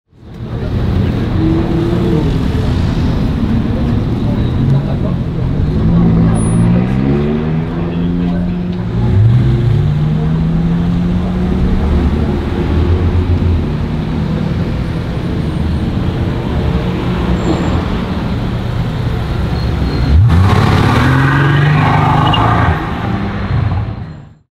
SUPERCARS Monaco - Mercedes Benz AMG SL65 Black Series
A Mercedes Benz AMG SL65 Black Series driving in Monaco.
What do you think of this car?